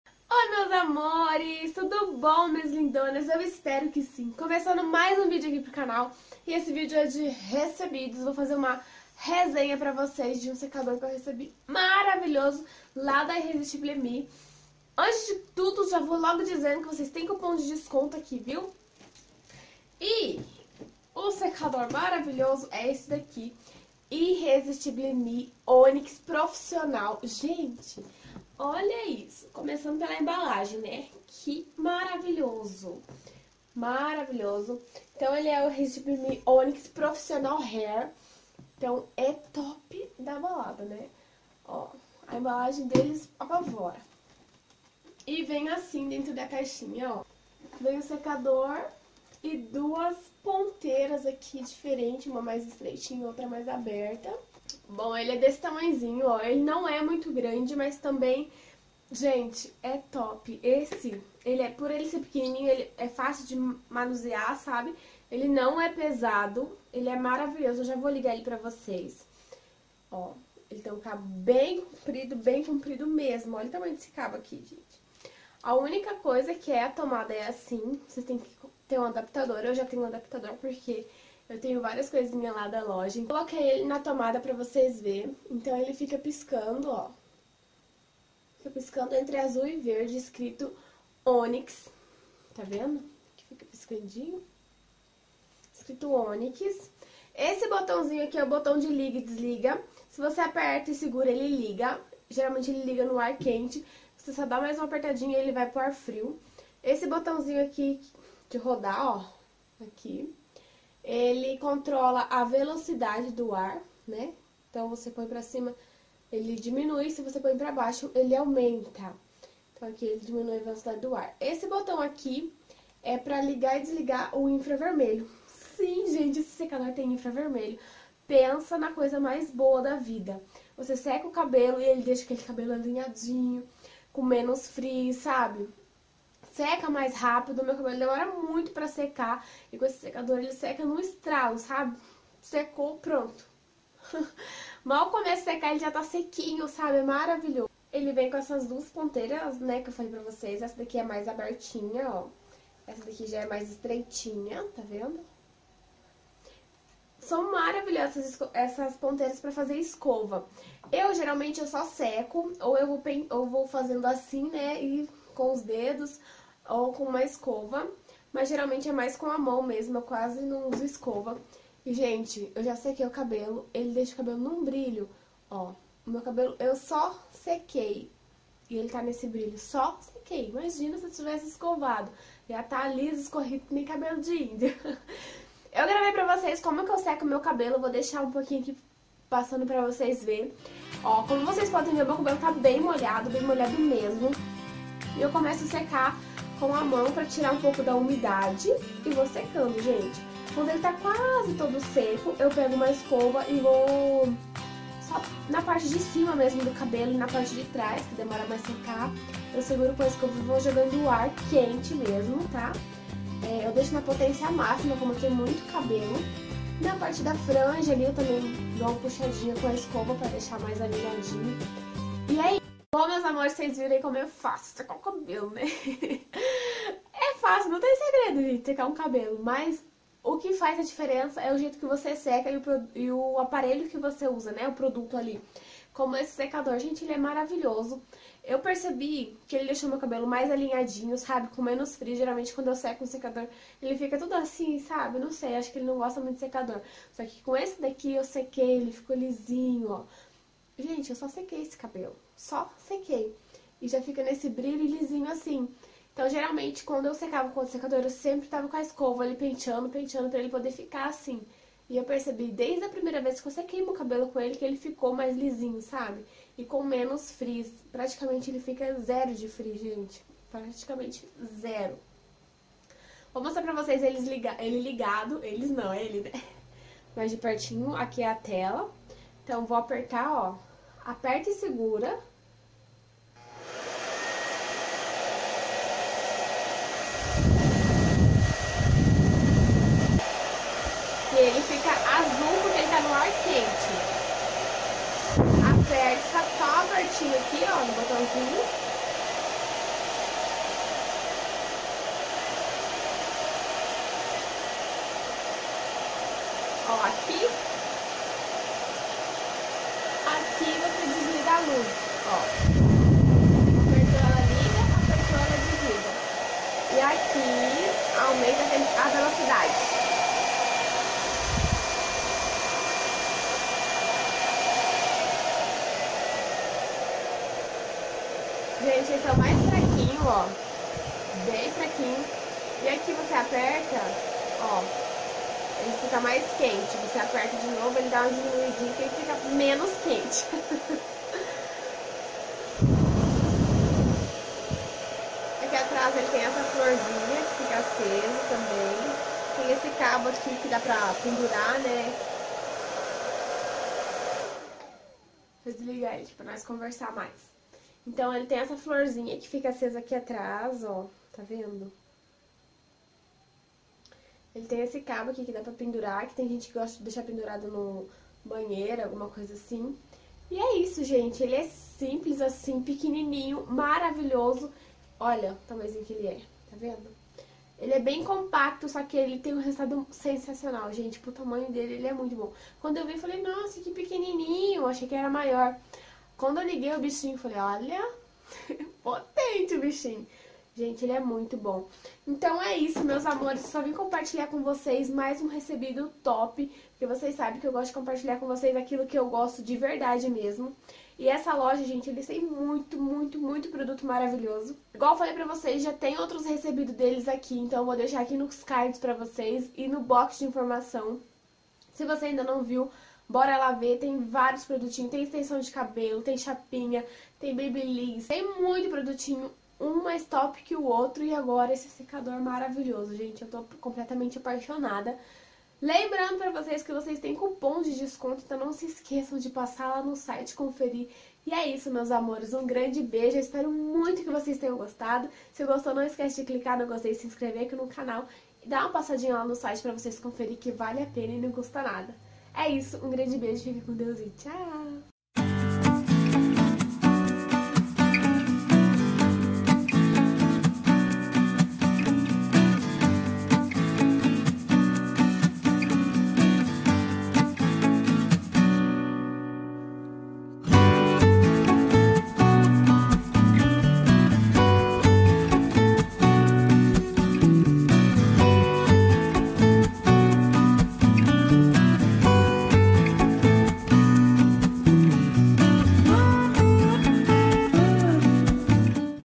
SECADOR COM INFRAVERMELHO|Onyx Professional Hair Dryer| RESENHA
Oii meus amores ♥
Hoje tem vídeo de resenha do secador de cabelo Onyx que recebia da loja Irresistible Me.